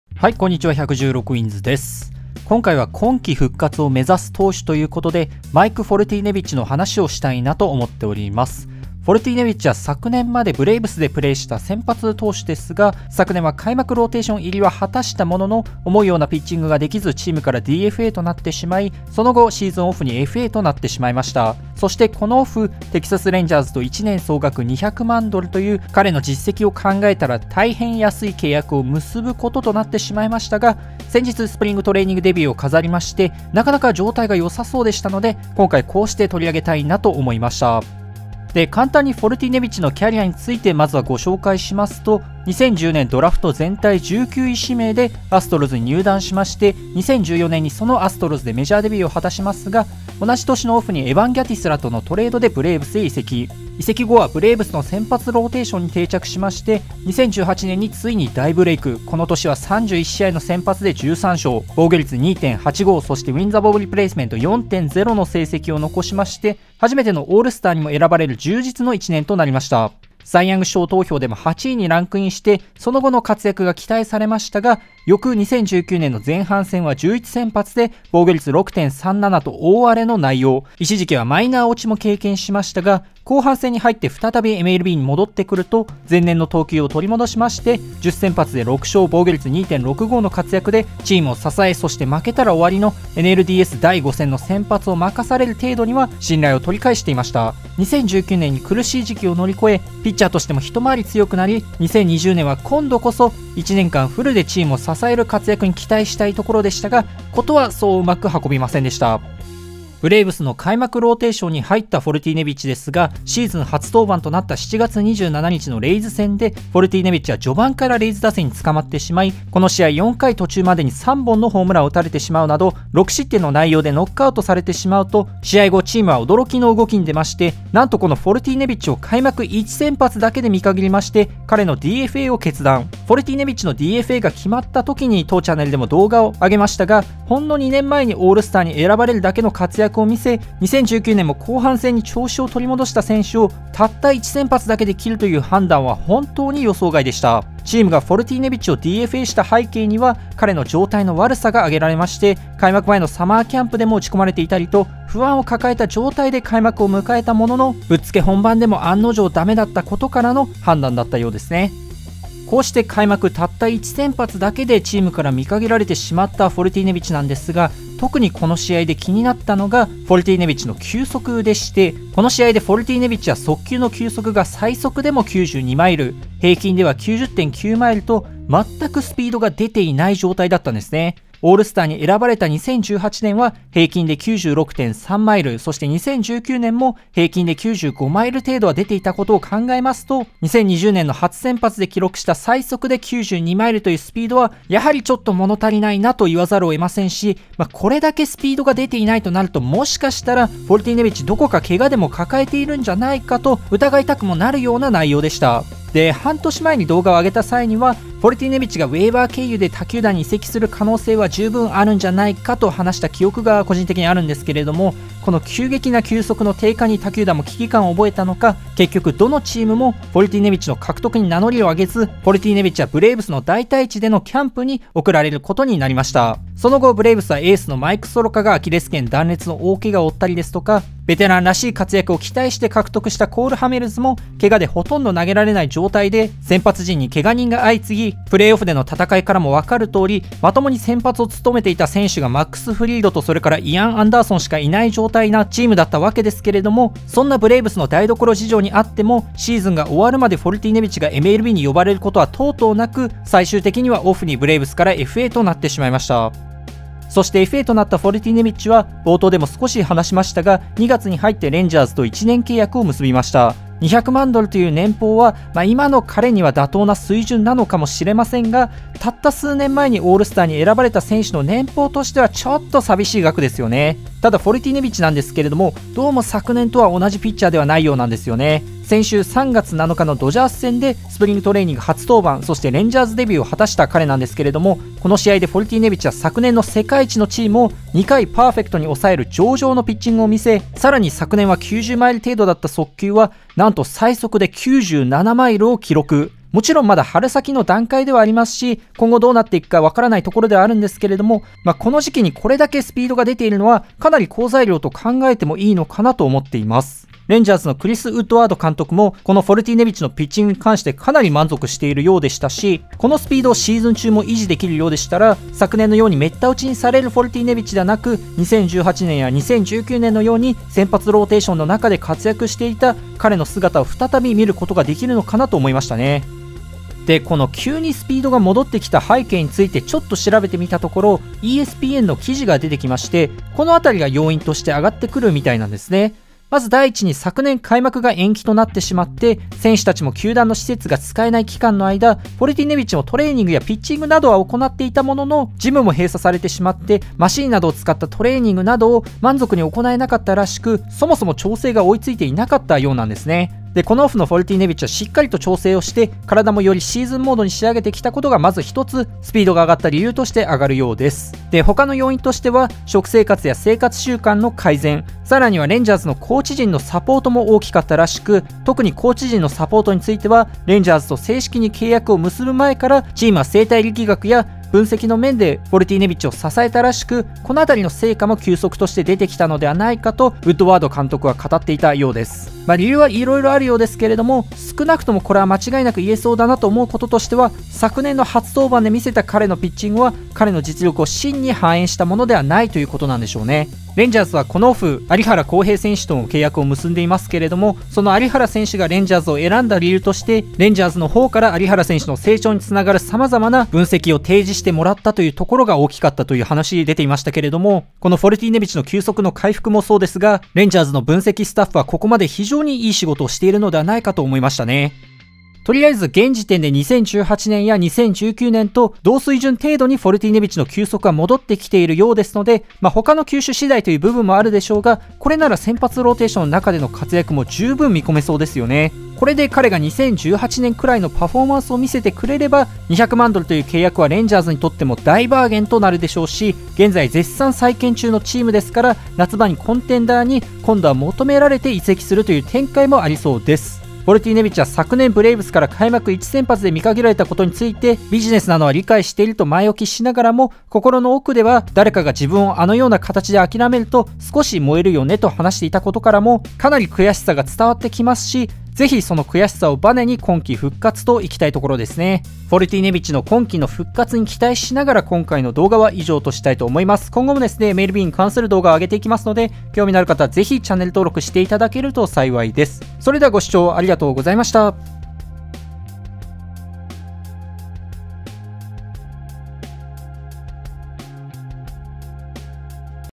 【MLB】開幕即戦力外の元球宴右腕フォルティネビッチが復活しそう
ちなみに無能ファンタジーベースボールプレイヤーこと116 Winsは、彼の球速が戻っていることを確認する前にフォルティネビッチをチームから落としたらしい。

2018年にオールスター&CY賞投票トップ10入りしたマイク・フォルティネビッチですが、昨年開幕1先発でブレーブスに見限られ、DFAされたのは記憶に新しいかと思います。その後FAとなった彼はオフにテキサス・レンジャーズと1年契約を結び、再起を図ろうとしています。

フォルティネビッチが見限られた要因として状態の悪さが挙げられまして、2018年は平均で96mphを記録していた速球が最速で92mphに落ち込んでしまい、ブレーブスとしてもこれでは厳しいという判断だったんでしょう。ただ、その判断はもしかしたら間違っていたのかもしれません。

※DFAは戦力外のみを意味する言葉ではありませんが、ここでは便宜上戦力外と表記しました。

■昨年彼のDFAが決まった際に上げた動画
【MLB】2018年オールスター&CY賞トップ10右腕フォルティネビッチが開幕初先発後に見限られた理由